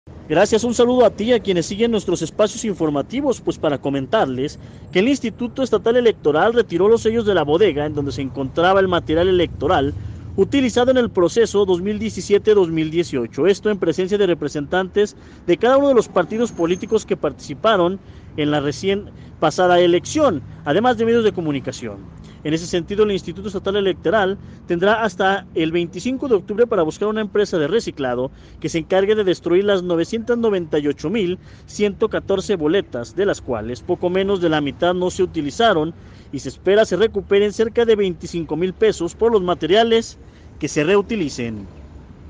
DESTRUYEN MATERIAL ELECTORAL
Se destruirán boletas y otros elementos utilizados en el pasado proceso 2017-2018.